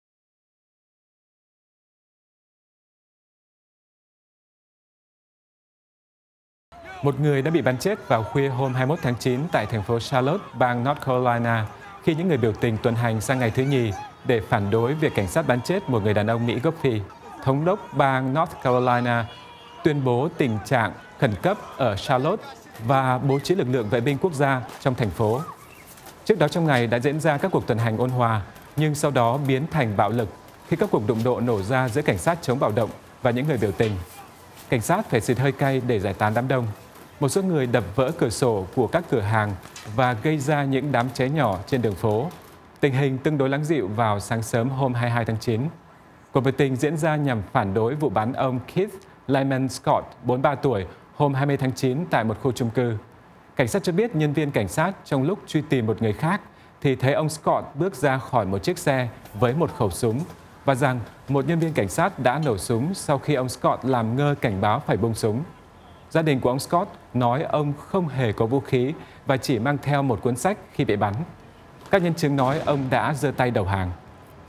Bạo động bước sang đêm thứ hai tại Charlotte
Tin tức Nếu không vào được VOA, xin các bạn hãy vào để vượt tường lửa. Một người đã bị bắn vào khuya 21/9 tại thành phố Charlotte, bang North Carolina, khi những người biểu tình tuần hành sang ngày thứ nhì để phản đối việc cảnh sát bắn chết một người đàn ông Mỹ gốc Phi.

Thống đốc bang North Carolina tuyên bố tình trạng khẩn cấp ở Charlotte và bố trí lực lượng Vệ binh Quốc gia trong thành phố.

Trước đó trong ngày đã diễn ra các cuộc tuần hành ôn hòa, nhưng sau đó biến thành bạo lực khi các cuộc đụng độ nổ ra giữa cảnh sát chống bạo động và những người biểu tình. Cảnh sát phải xịt hơi cay để giải tán đám đông. Một số người đập vỡ cửa sổ của các cửa hàng và gây ra những đám cháy nhỏ trên đường phố.

Tình hình tương đối lắng dịu vào sáng sớm 22/9.

Ban đầu, chính quyền thành phố nói có một người thiệt mạng vào đêm 21/9, do một thường dân chứ không do cảnh sát bắn, nhưng sau đó cho biết nạn nhân bị thương trầm trọng. Sở cảnh sát Charlotte cho biết bốn nhân viên của họ bị thương, nhưng không ai bị đe dọa đến tính mạng.

Hôm 20/9, đêm đầu tiên của cuộc biểu tình, có khoảng 24 người bị thương, trong đó có 16 cảnh sát.

Thị trưởng Jennifer Roberts kêu gọi người dân bình tĩnh, nói rằng Charlotte luôn luôn là nơi mà mọi người có thể đối thoại ôn hòa.

Cuộc biểu tình diễn ra nhằm phản đối vụ bắn ông Keith Lamont Scott, 43 tuổi, hôm 20/9 tại một khu chung cư. Cảnh sát cho biết nhân viên cảnh sát trong lúc truy tìm một người khác thì thấy ông Scott bước ra khỏi một chiếc xe với một khẩu súng, và rằng một nhân viên cảnh sát đã nổ súng sau khi ông Scott làm ngơ cảnh báo phải buông súng.

Gia đình của ông Scott nói ông không hề có vũ khí, và chỉ mang theo một cuốn sách khi bị bắn. Các nhân chứng nói ông đã giơ tay đầu hàng.

Các camera trên người của cảnh sát đã quay được cảnh nổ súng, nhưng đoạn video này chưa được công bố.

Thị trưởng Roberts nói bà sẽ cùng với các lãnh đạo cộng đồng xem đoạn video này trong ngày 22/9.

Đây là vụ mới nhất trong một chuỗi vụ nổ súng của cảnh sát đã làm nổi cộm vấn đề cảnh sát sử dụng vũ lực, đặc biệt là đối với người Mỹ gốc Phi.

Tổng thống Barack Obama đã nói chuyện qua điện thoại với bà Roberts và Thị trưởng Dewey Bartlett của thành phố Tulsa, Oklahoma, nơi một nhân viên da trắng bắn chết một người đàn ông da đen hôm 16/9.

Tòa Bạch Ốc nói sau cuộc đối thoại, cả ba khẳng định rằng bất kỳ cuộc biểu tình nào cũng phải ôn hoà, và tổng thống hứa cung cấp bất kỳ sự hỗ trợ cần thiết nào cho cả hai thành phố.

Ứng viên tổng thống của đảng Cộng hòa Donald Trump viết trên trang Twitter rằng “tình trạng ở Tulsa và Charlotte thật bi thảm. Chúng ta phải cùng nhau làm cho nước Mỹ an toàn trở lại”. Ông nói ông hy vọng bạo lực và bất ổn ở Charlotte sẽ chấm dứt ngay lập tức.

Tờ Washington Post đang thực hiện một dự án theo dõi số lượng người bị cảnh sát Mỹ giết chết. Nguyên nhân thực hiện dự án này là do thiếu một hồ sơ dữ liệu hoàn chỉnh trong cơ sở dữ liệu của chính phủ liên bang. Tính đến ngày 22/9, tờ Washington Post đã ghi nhận có 706 người bị bắn chết bởi cảnh sát vào năm 2016, trong đó có 173 người Mỹ gốc Phi.